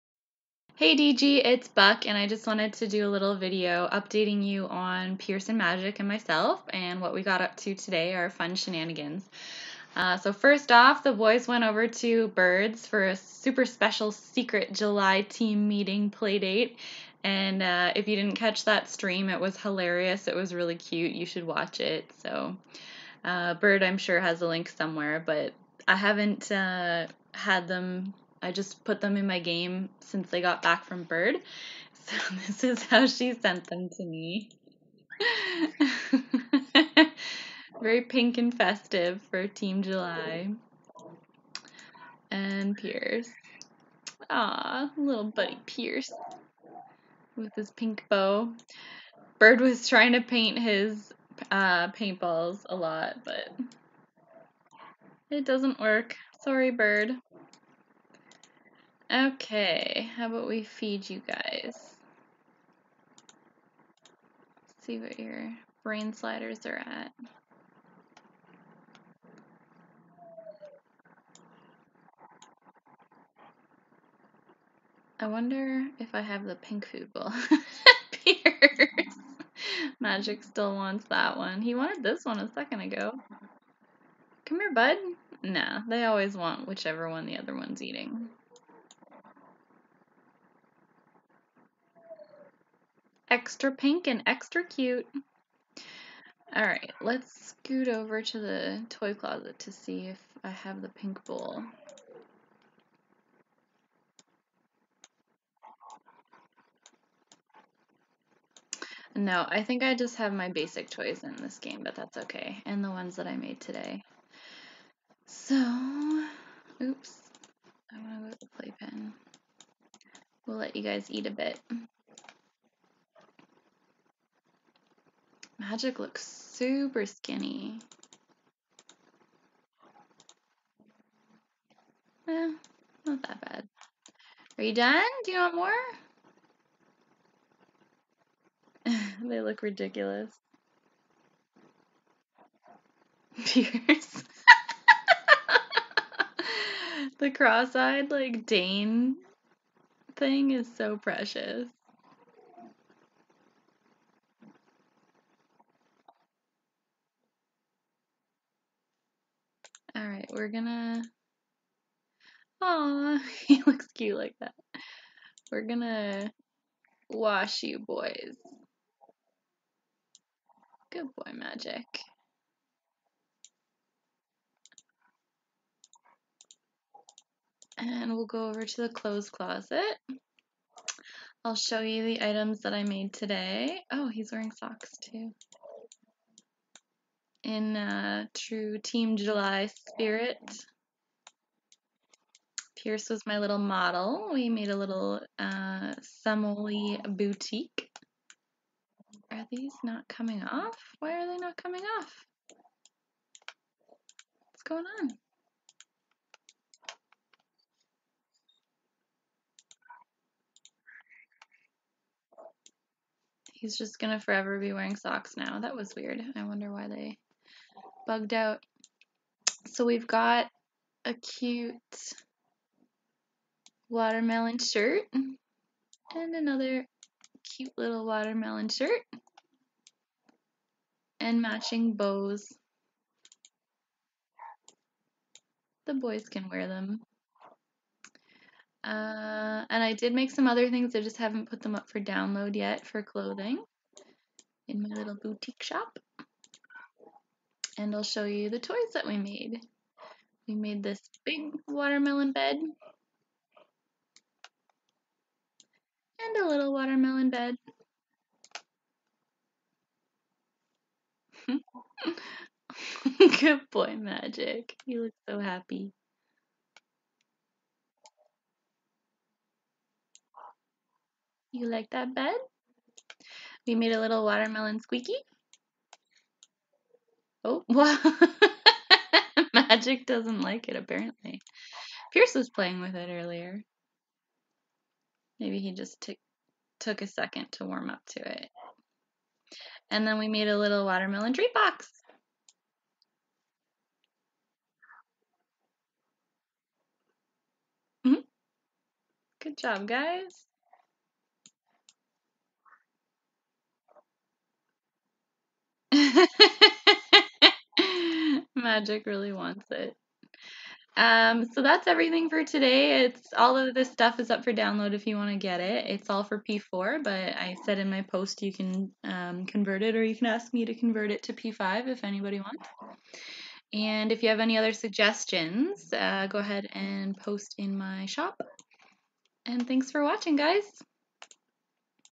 PearceMagicUpdateAug19
Pearce and Magic came home from bird's place a mess (but not pregnant lol) and then we show off what we got up to today!